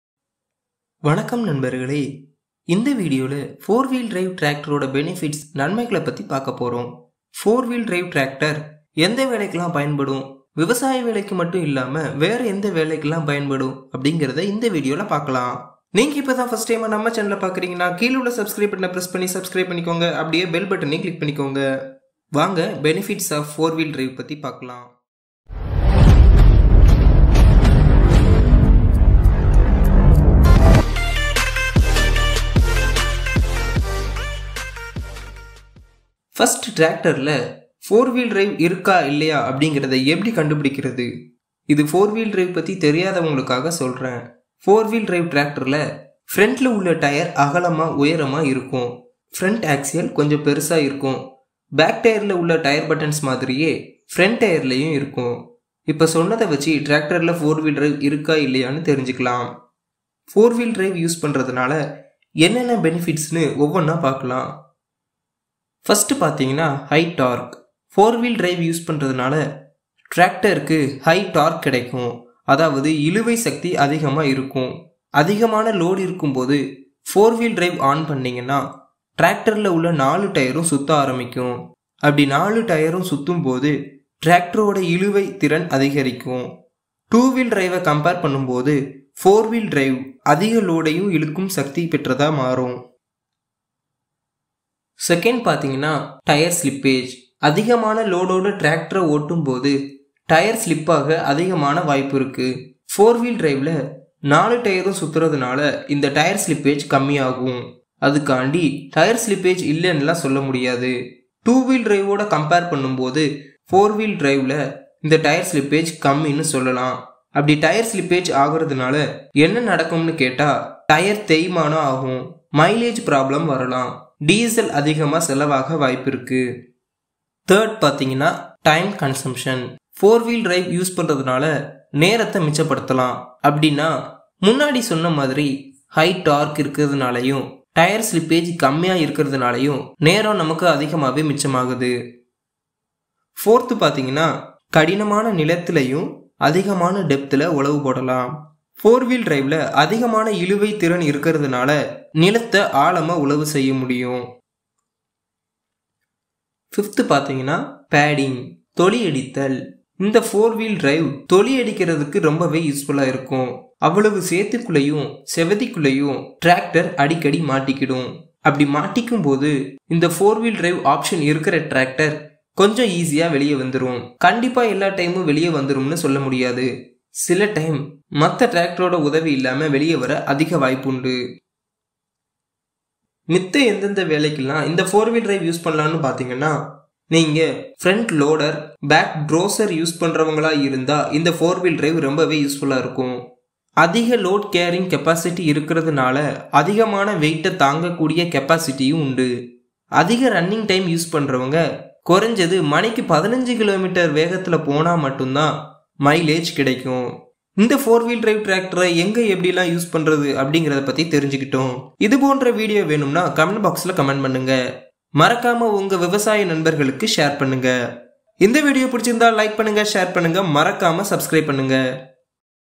Benefits Of 4WD Tractor over 2WDTractor | Tamilians Agro
Benefits of 4wd tractor is discussed in this video and more over where 4wd tractor is use and what type of work can do in agriculture and also other than in non agriculture work is talked in this video.
MORE DETAILS ABOUT TRACTOR
Shop on online Amazon (support us:)
Daily Deal of the Day are listed ( best offered products) in Groups
To Join: